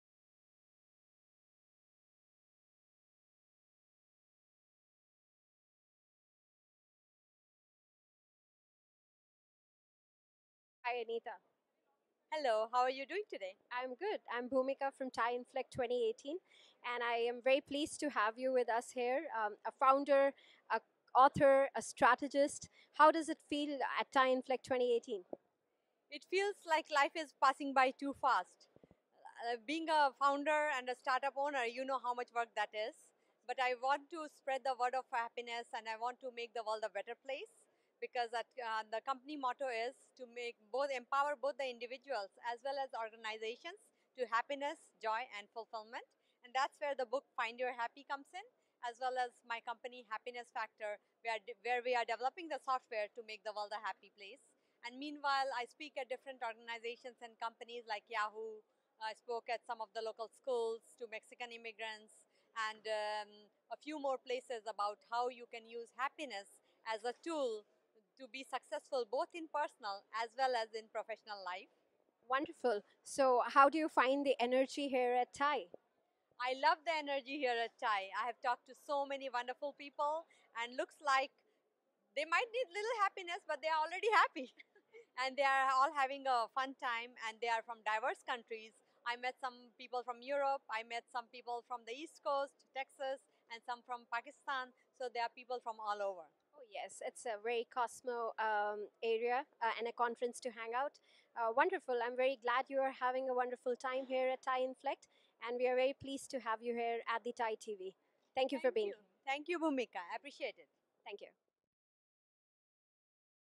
Anita Kumari's @TiEInflect 2018 Interview
Watch Anita Kumari's (Founder of Strategies Happiness) Interview at the TiETV Media Lounge at @TiEinflect 2018. cc